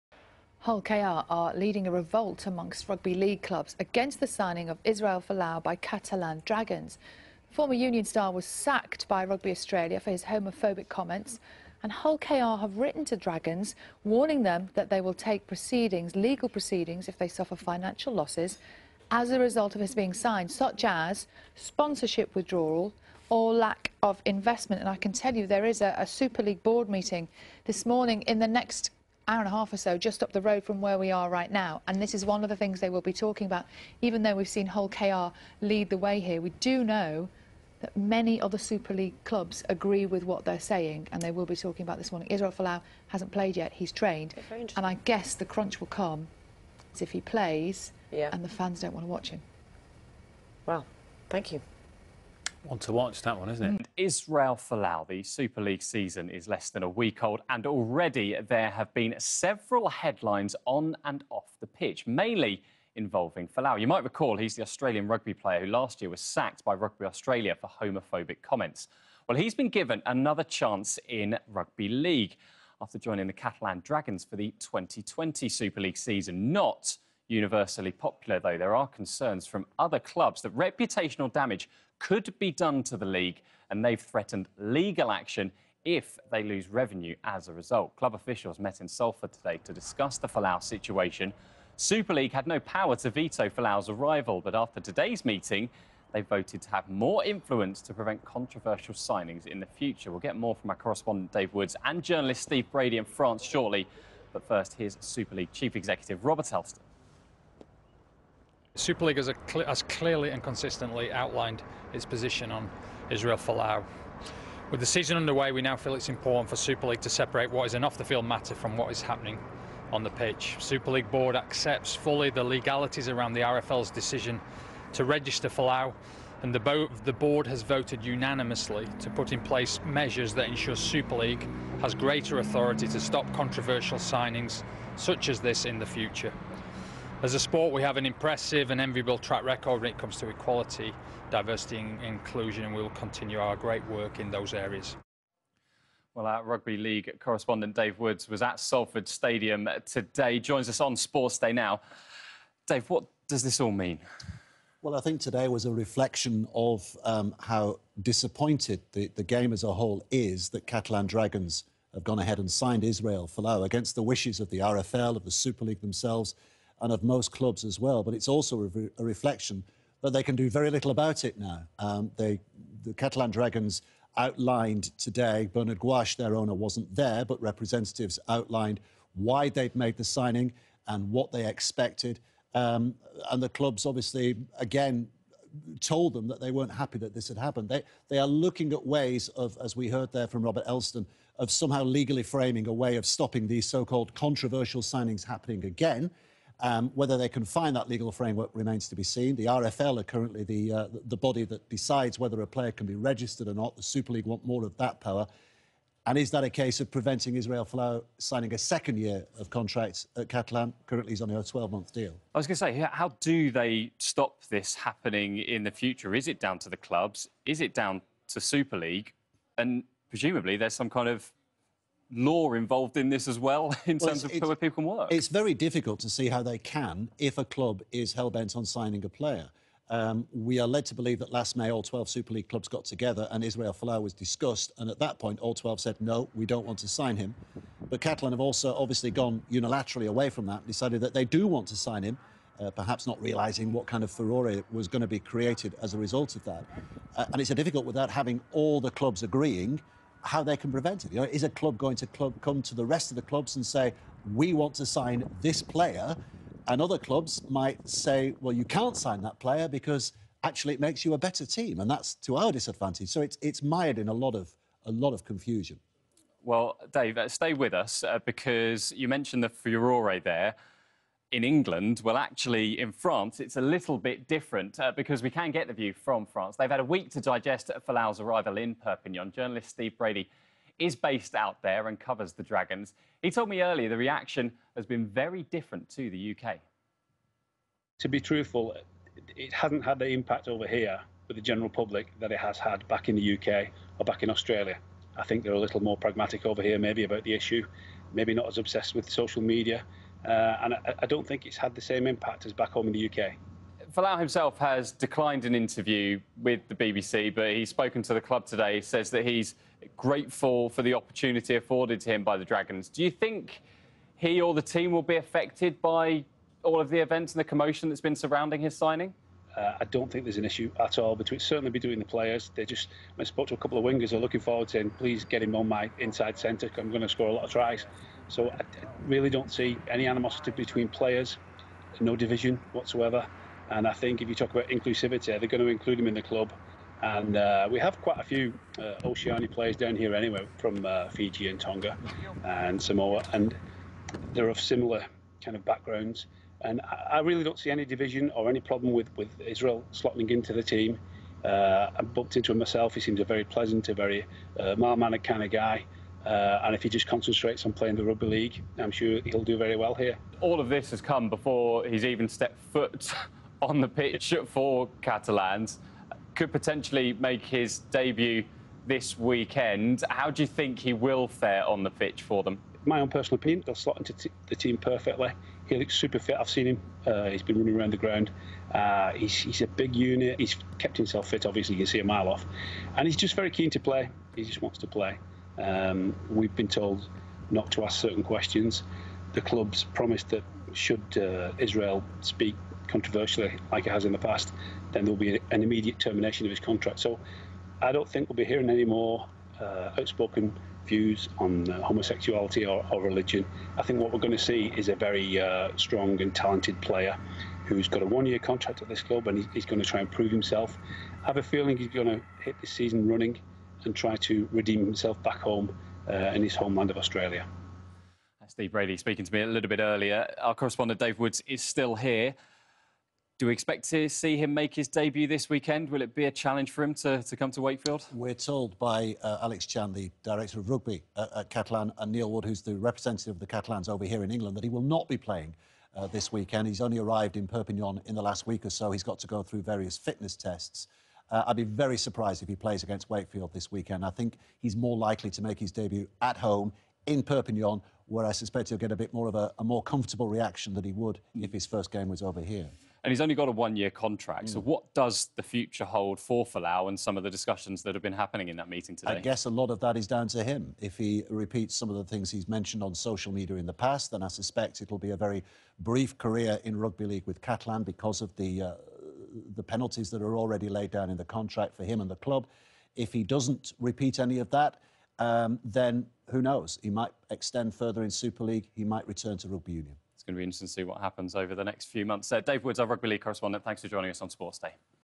Revolt over Israel Folau Signing for Catalans Dragons because "he's a homophobic Christian!!!"
The Rugby Super League has unanimously voted to give itself greater authority to stop future "controversial signings" such as Israel Folau's move to Catalans Dragons.